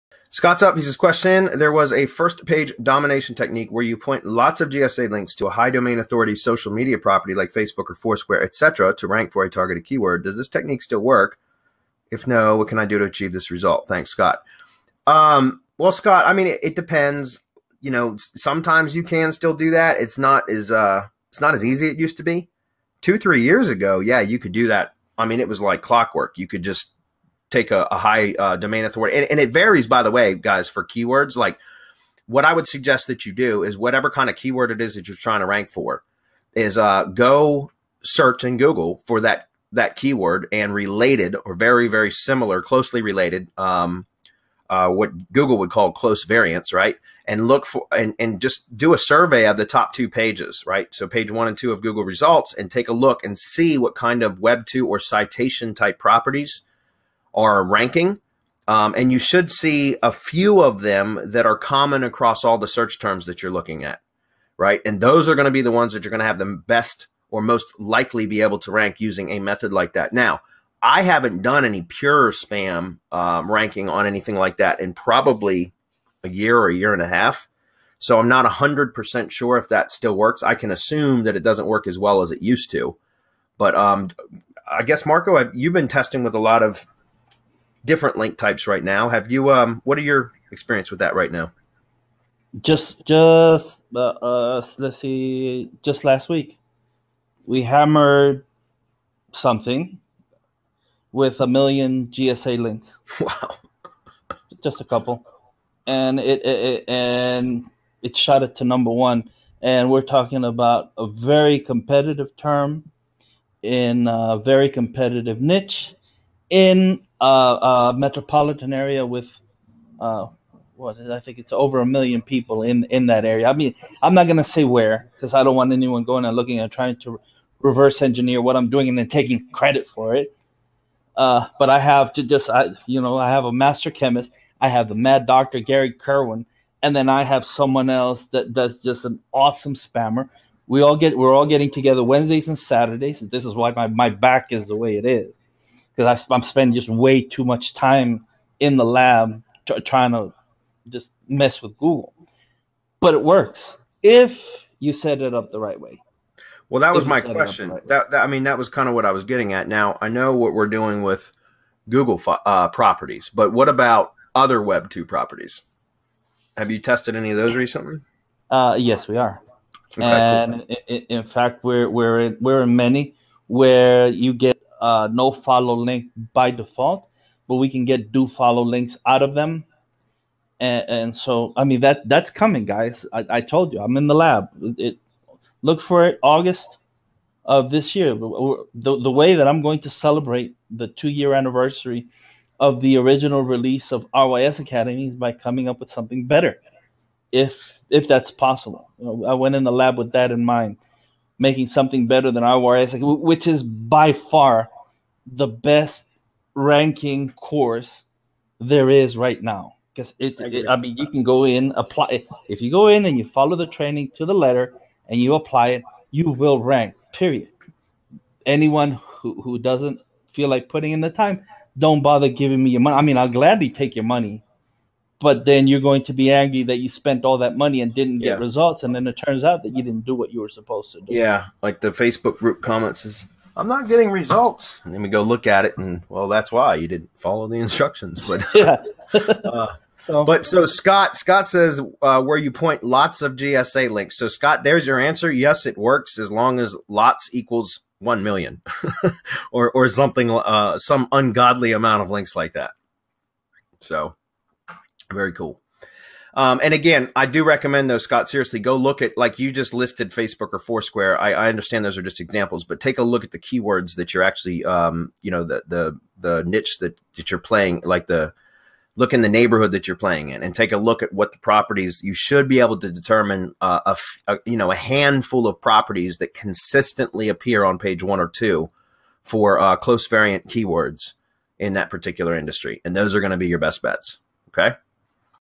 Does The First Page Domination Technique Still Work?
In episode 121 of Semantic Mastery's weekly Hump Day Hangouts one viewer asked if the First Page Domination SEO technique still works.

The exact question was:

There was a 1st page domination technique where you point lots of GSA links to a high Domain Authority social media property like Facebook or Foursquare etc. to rank for a targeted keyword. Does this technique still work? If no what can I do to achieve this result?